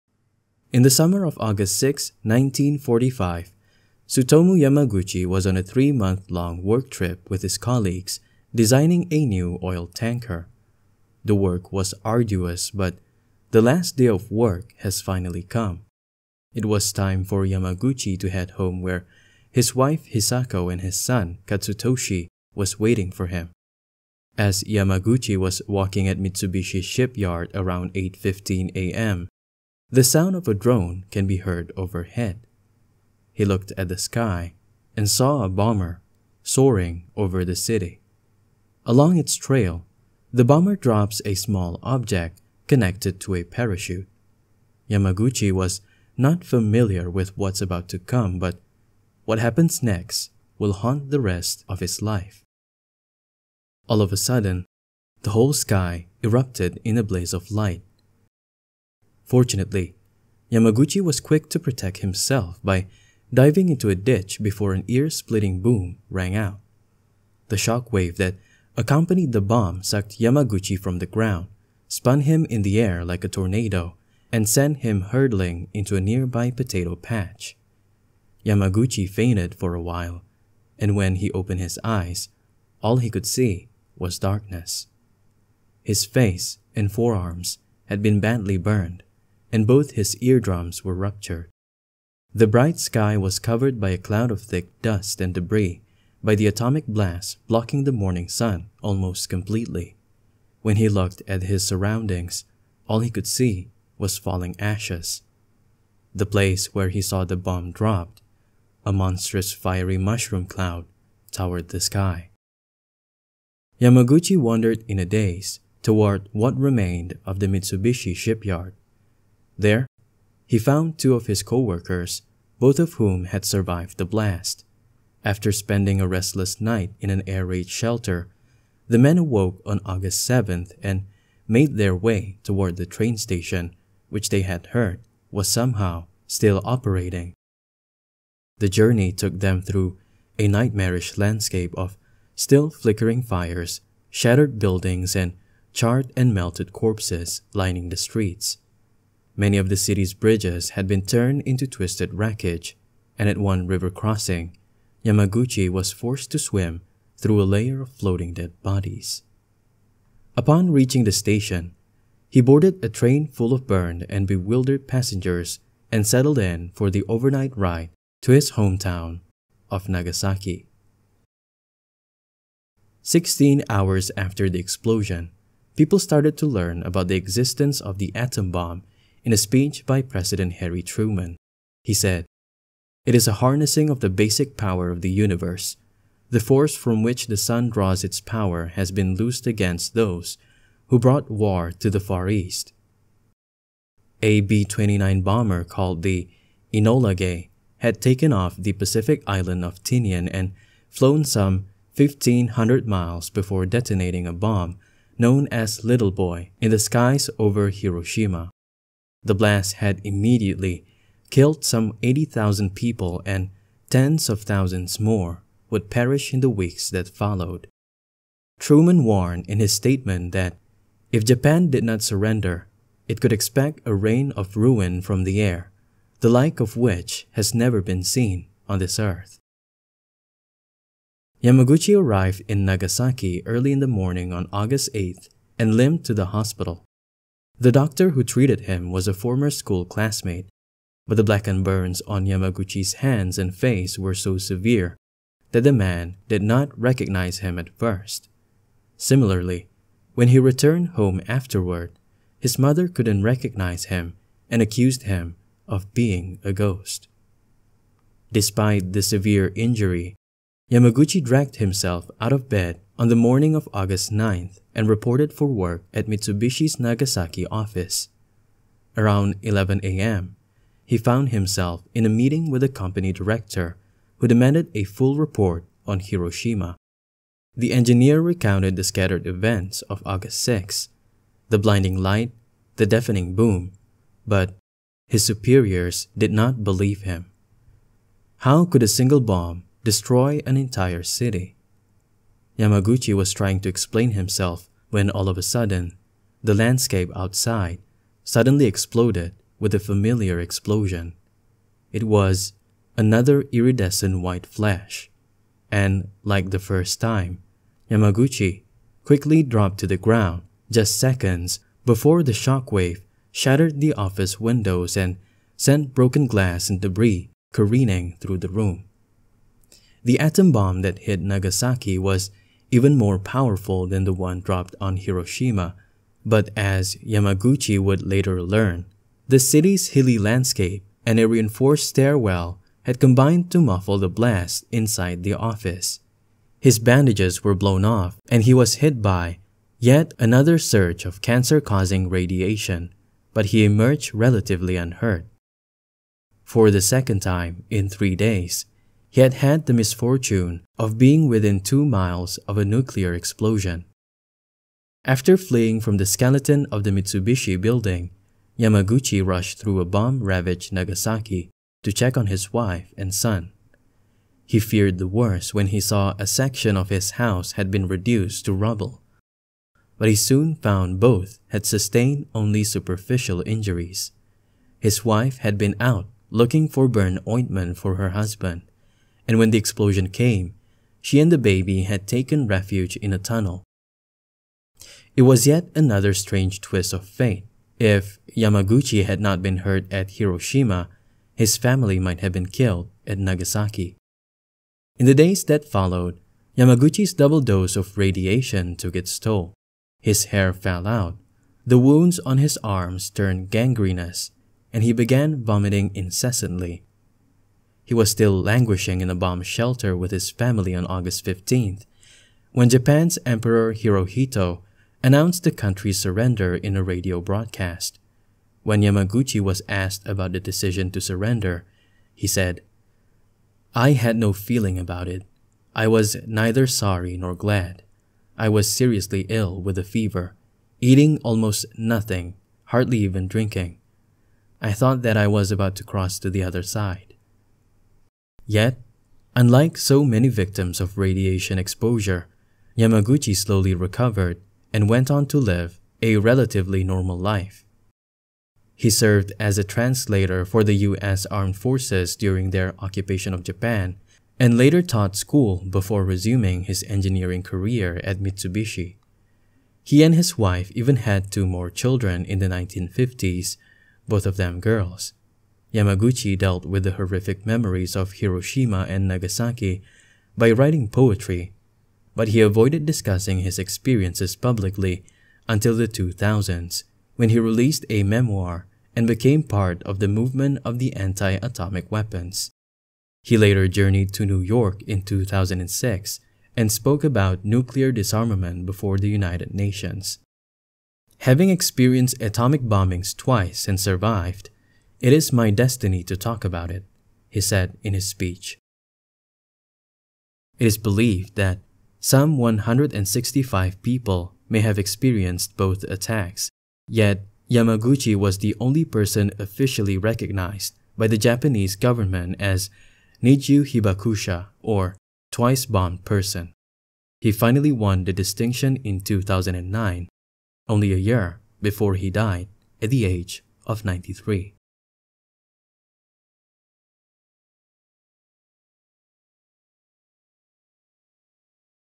The Man Who Survived Two Nuclear Bombs - Tsutomu Yamaguchi
This is the incredible story of the man who survived two nuclear bombs - Tsutomu Yamaguchi. The first nuclear bomb that Yamaguchi endured happened at Hiroshima on August 6th 1945. He was on a 3-month long work trip where on that morning he saw a bomber flying over the city and on its trail, a small object can be seen with a parachute. Yamaguchi was not familiar with what’s about to come but what happens next will haunt the rest of his life.

He incredibly survived the first nuclear bomb even though he was badly burned. As soon as he woke up from the blast, he went on a train full of burned and bewildered passengers and settled in for the overnight ride to his hometown of Nagasaki. This is where he endured the second nuclear blast...

Timestamps:
Hiroshima nuclear blast - 0:00
Radio broadcast - 2:52
Nagasaki nuclear blast - 3:50
Aftermath - 6:33